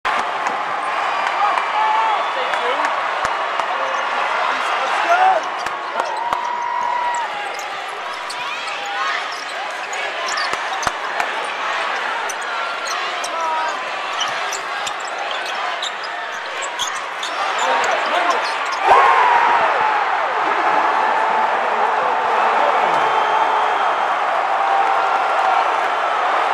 Ronald Moore: Another Huge Three
Ronald Moore nails a three late in the second half to get the crowd going in Siena's eventual overtime victory in the MAAC Championship game over the Fairfield Stags.